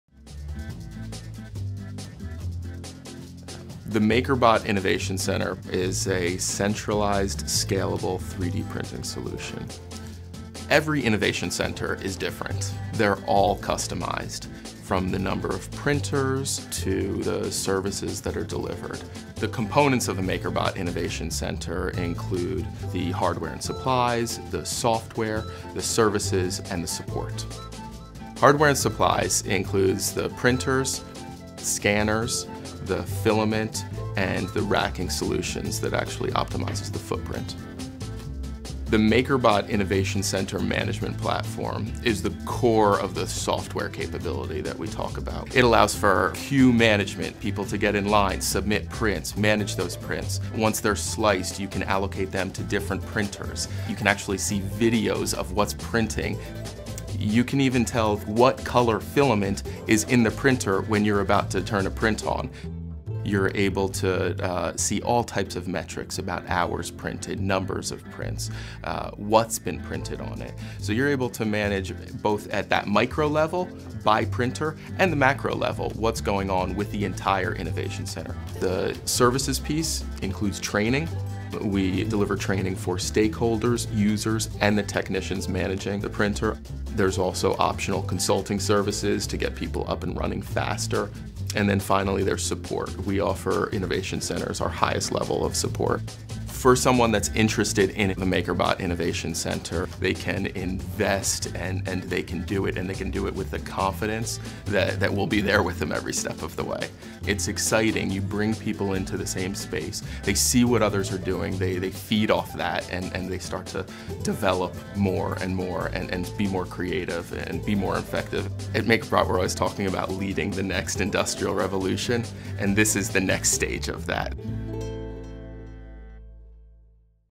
From the Makers | MakerBot Innovation Centers
Join MakerBot VP of Professional Services Jordan Brehove as he discusses MakerBot Innovation Centers. A MakerBot Innovation Center is a centralized, scalable 3D printing solution composed of MakerBot Replicator 3D printers, software, supplies, and services.